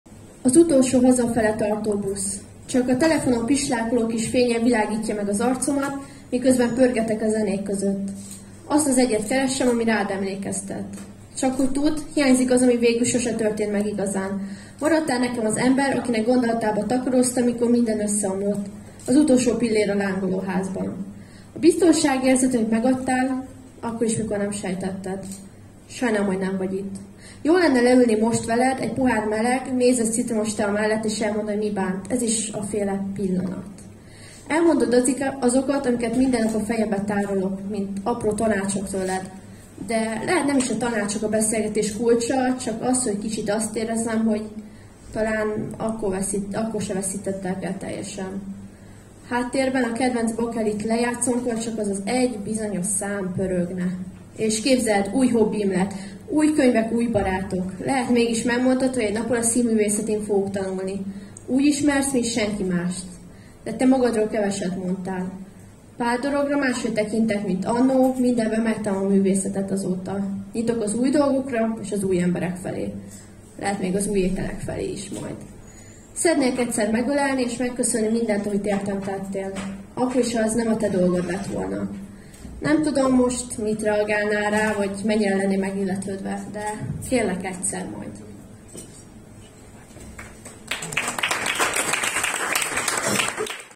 (Pillanatkép témaverseny) Pongrácz Eszter
odaLÖKött KÖLtészet - Slam Poetry Veszprém
2023 decemberi Slam Poetry Klub
FOTON Audiovizuális Centrum, Veszprém, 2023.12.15.

Video copyright: Tóth Roland
Grafika: Nagy Bianka